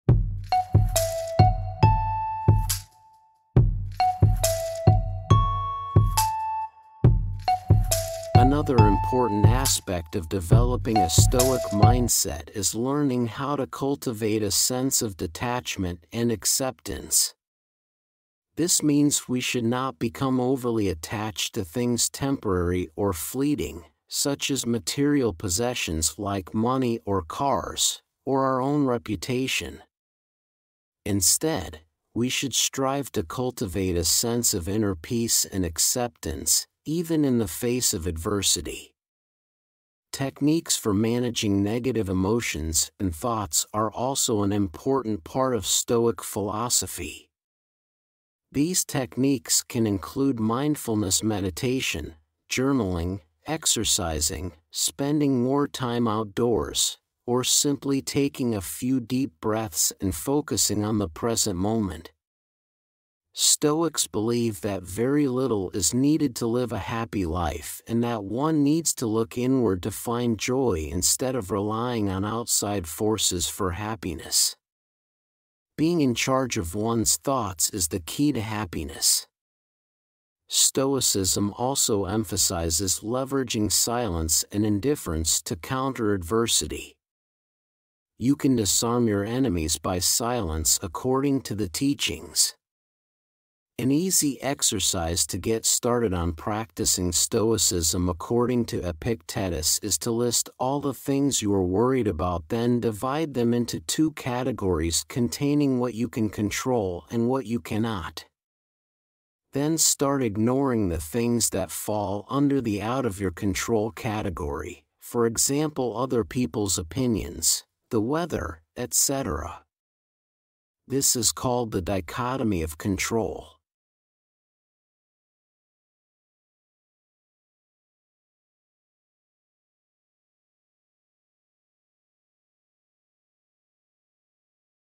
Stoicism 101: A Beginner's Guide
Beginner guide to Stoicism - Subscribe to get daily Stoic Videos and learn how you can apply Stoic practices in your daily life to create changes in how you deal with your Co-workers, Family and others that you may relate to as you go by your day. Practice focusing on what is under your control and letting go of what is not. Let's walk together on this journey of becoming better versions of ourselves by applying Stoic lessons from Philosophers like Marcus Aurelius, Seneca, Epictetus and others.